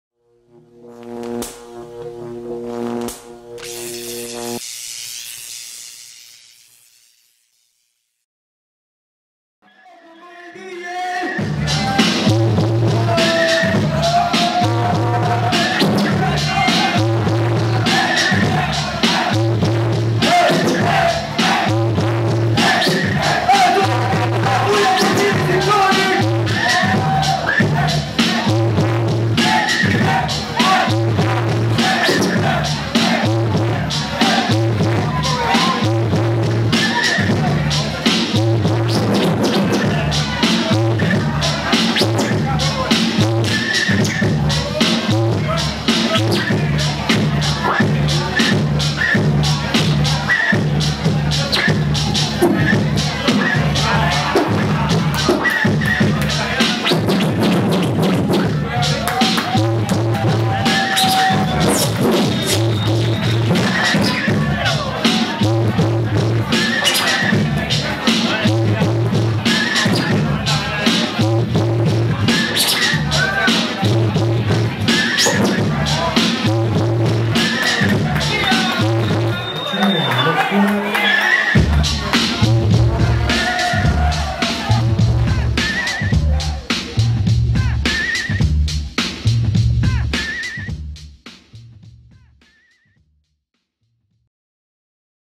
Dj Cidtronyck Beat Juggling (en vivo ZKT1 Audiovisual)
Dj Cidtronyck Beat Juggling en vivo.
07 / 01 / 2012. Melipilla, evento Return of the Boombap.